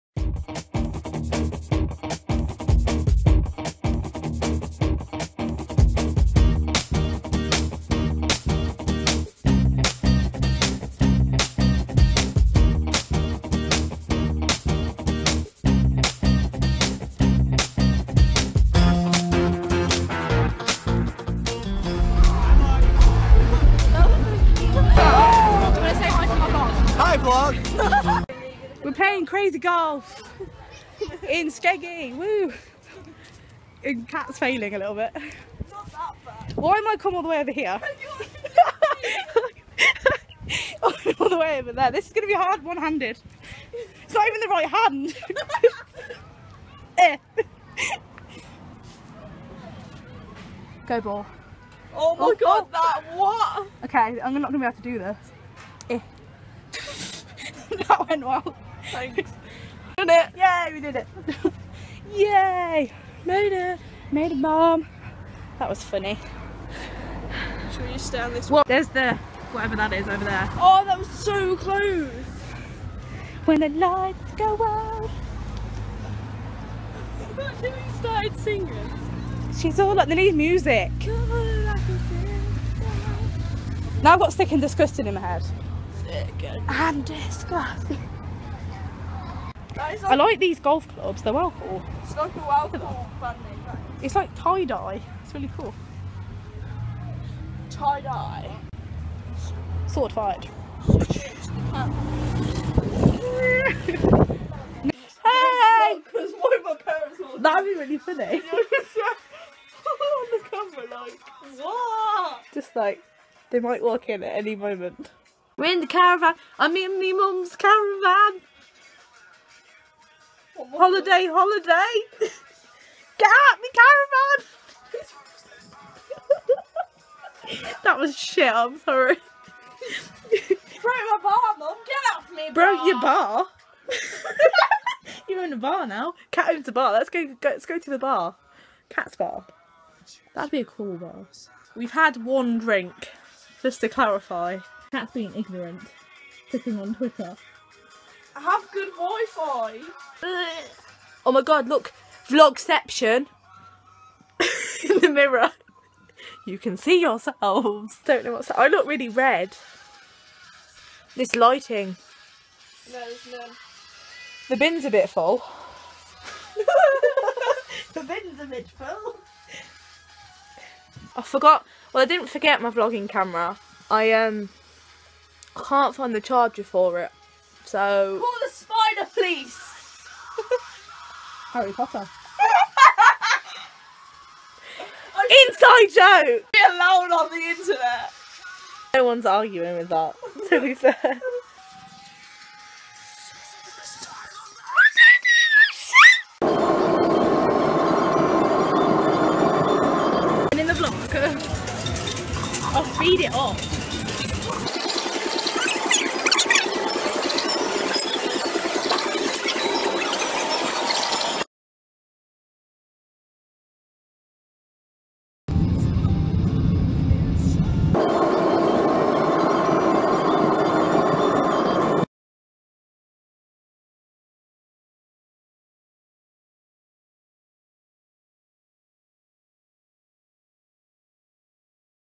Vlogging in Skegness
Me and Catt took a trip to Skegness with her parents over the weekend and this is a short vlog I made while we were there.